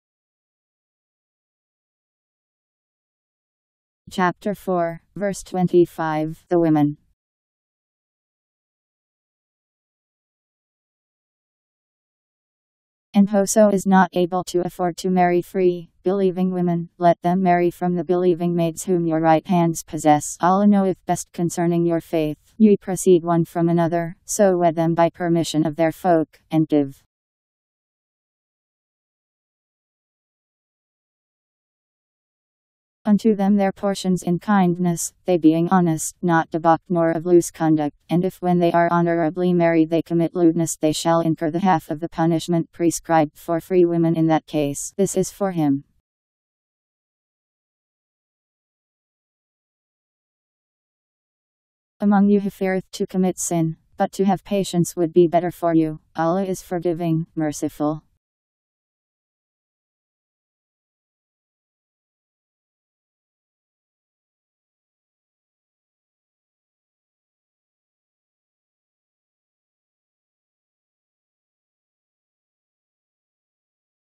Quran The Women 4:25
Video shows Quran The Women Chapter 4 Verse 25: And whoso is not able to afford to marry free, believing women, let them marry from the believing maids whom your right hands possess. Allah knoweth best (concerning) your faith. Ye (proceed) one from another; so wed them by permission of their folk, and give unto them their portions in kindness, they being honest, not debauched nor of loose conduct. And if when they are honourably married they commit lewdness they shall incur the half of the punishment (prescribed) for free women (in that case). This is for him among you who feareth to commit sin. But to have patience would be better for you. Allah is Forgiving, Merciful. Powered by MaryTTS. Holy Qur'an 4:25 Surah : Ayah